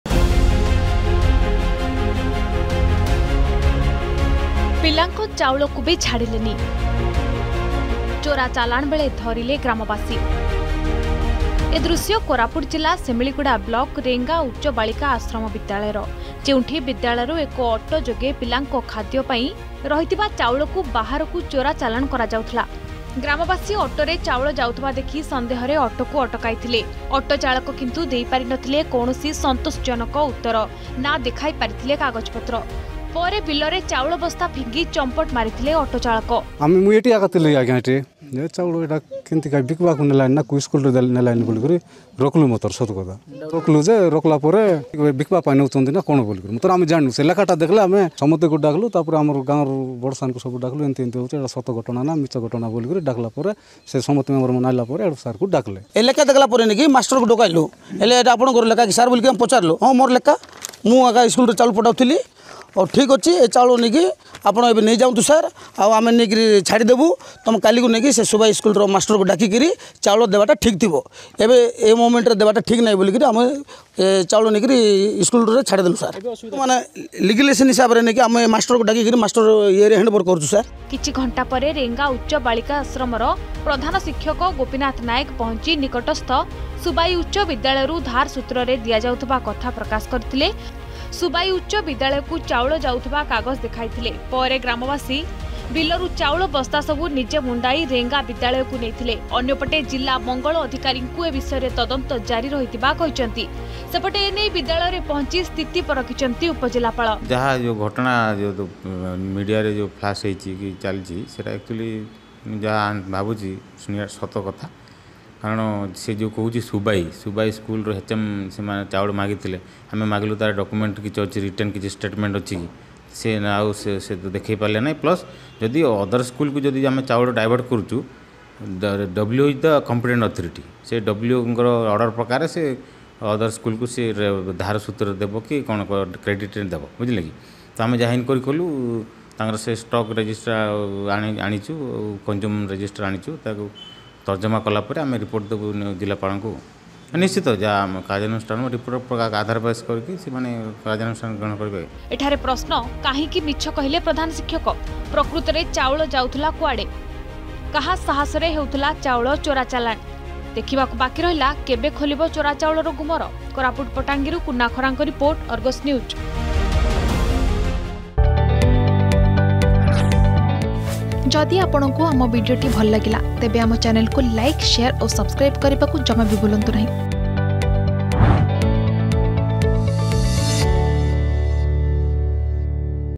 Villagers Expose Head Master For Stealing Mid-Day Meal Rice In Odisha’s Koraput
Villagers Expose Head Master For Stealing Mid-Day Meal Rice In Odisha’s Koraput. This comes to fore from Renga Girl's High School. Further investigation into the matter is still on.